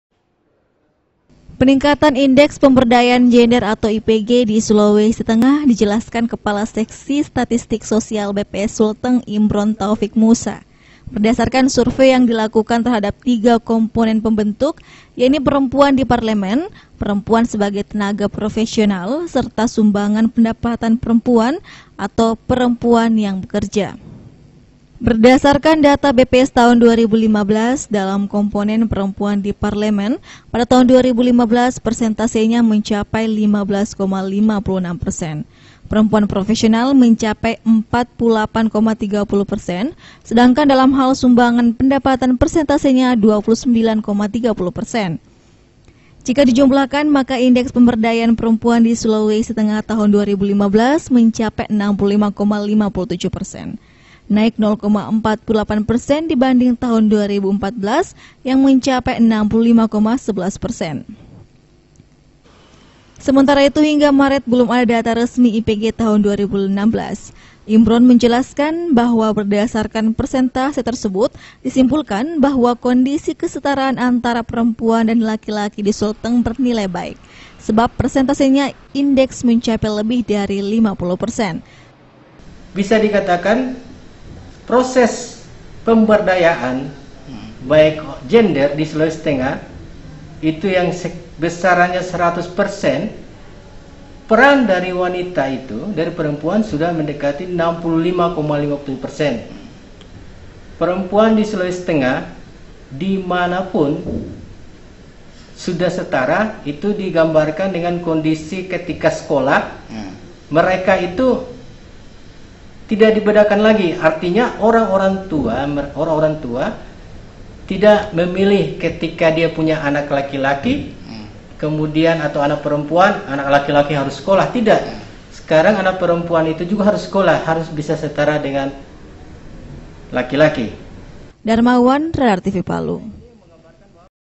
Indeks Pemberdayaan Gender ( Perempuan ) di Sulteng Mambaik
Berdasarkan data dari Badan Pusat Statistik ( BPS ) Provinsi Sulawesi Tengah, hingga tahun 2015 Indeks Pemberdayaan Gender di Sulawesi Tengah mengalami peningkatan 0,48 persen atau mencapai 65,57 persen.